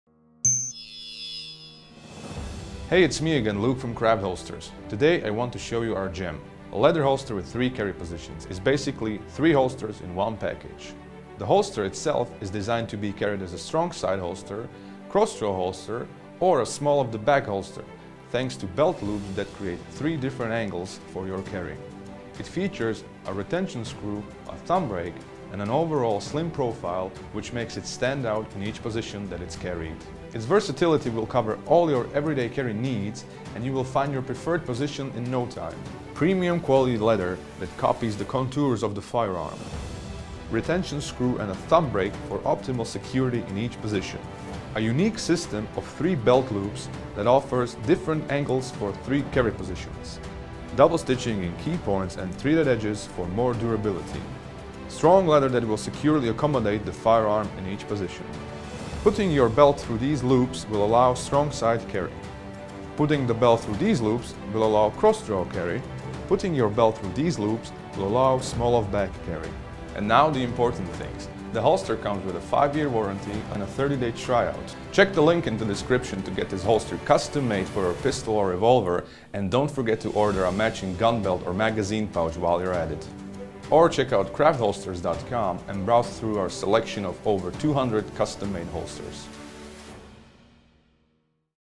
Leather Holster w 3 Carry Positions l Craft Holsters Review
If you are looking for a versatile holster for your daily whereabouts this is the one for you. It’s best used as a SOB holster where it exceeds, but can be also worn on your strong side or cross-draw. You can use it on the range, in your car or just as a daily carry piece. The holster is tightly sitting on the belt thanks to a clever belt loop system with multiple openings that create the three angles of the carry and offers fast draw thanks to a quick release clip.

Remember to stay safe and find your perfect fit.